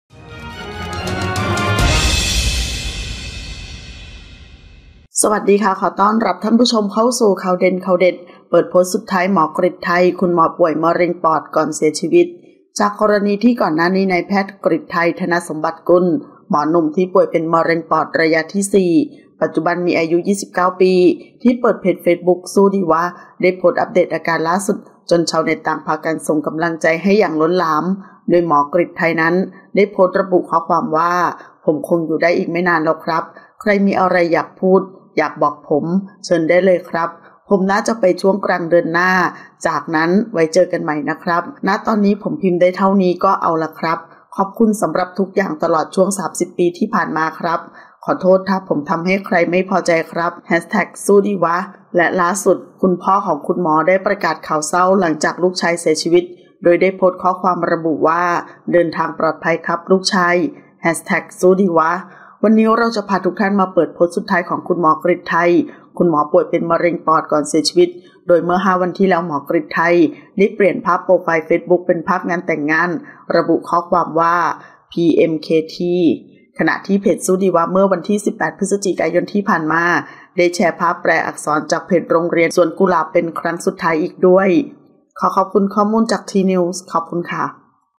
เปิดโพสต์สุดท้าย “หมอกฤตไท” คุณหมอป่วยมะเร็งปอด ก่อนเสียชีวิต
00:00 - Intro
00:10 - News
01:00 - Minute